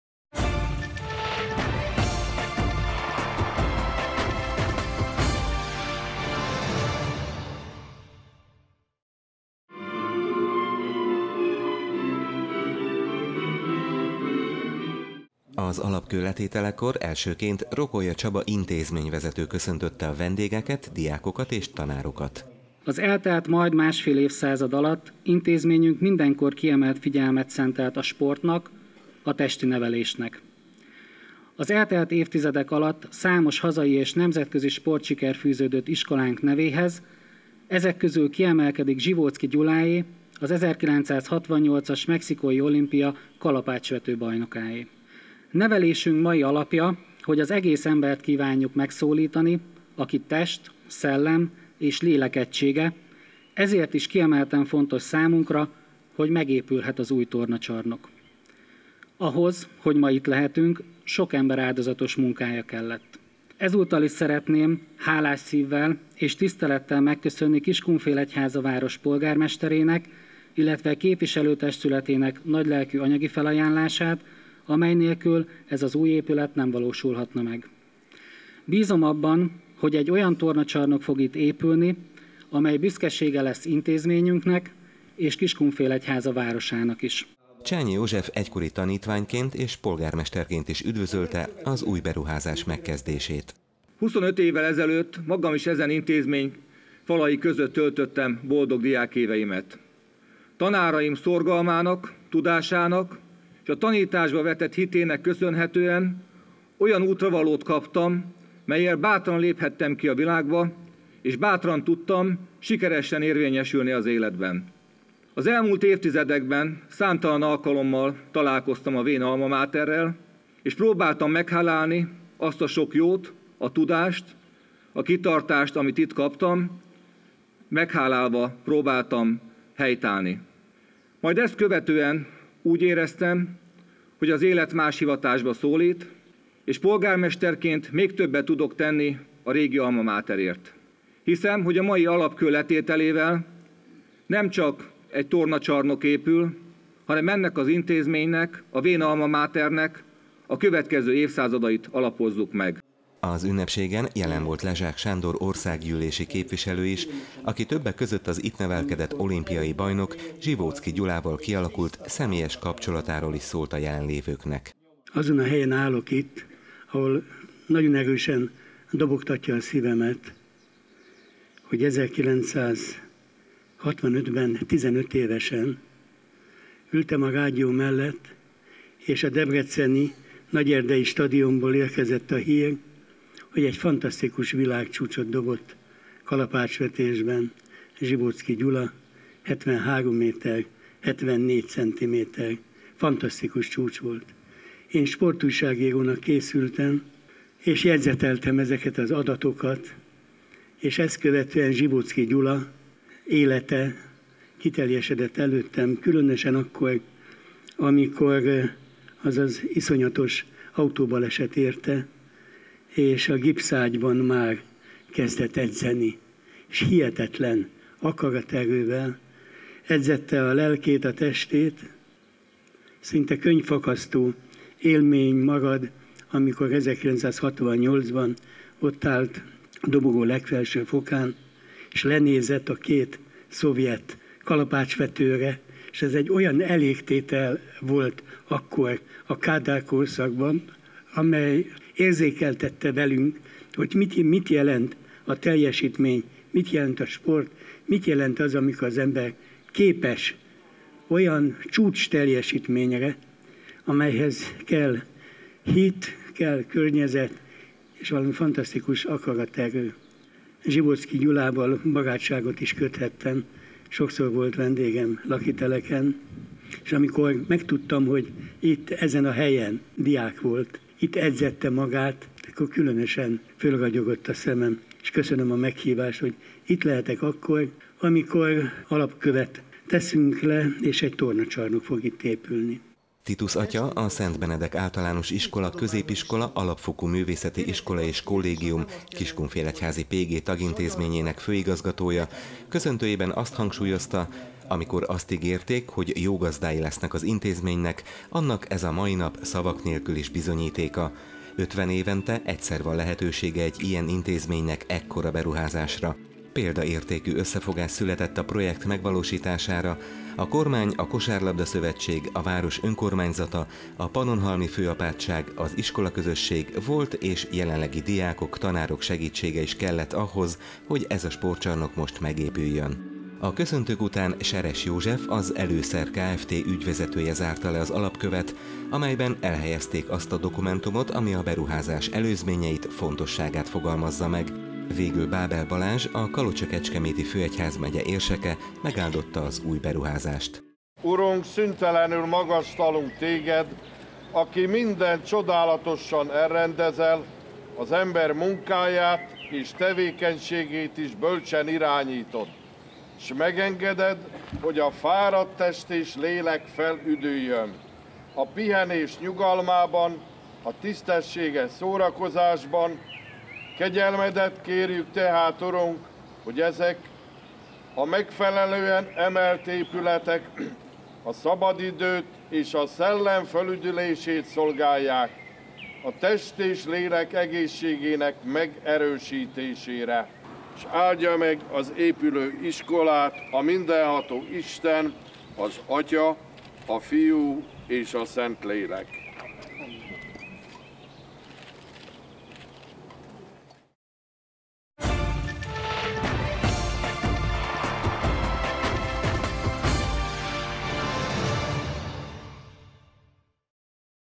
Új sportcsarnok épül a PG-ben - 2017.04.28.
Január elején elbúcsúztatták , majd hamarosan le is bontották a PG 112 éve épült tornatermét. Április 28-án azonban már egy új, korszerű sportcsarnok alapkőletételére került sor. – Az eltelt másfél évszázad alatt intézményünk mindenkor kiemelt figyelmet szentelt a sportra, a testnevelésre. Az elmúlt évtizedek során számos, nemzetközileg is sikeres sportoló nevelkedett itt.

Közülük kiemelkedik Zsivótzky Gyula, aki 1968-ban a mexikói olimpián kalapácsvetésben aranyérmet szerzett. Nevelésünk alapelve ma is az, hogy az egész embert kívánjuk megszólítani, aki test, szellem és lélek egysége. Ezért is kiemelten fontos számunkra, hogy megépülhet új tornacsarnokunk – mondta az alapkő elhelyezése előtt Rokolya Csaba intézményvezető.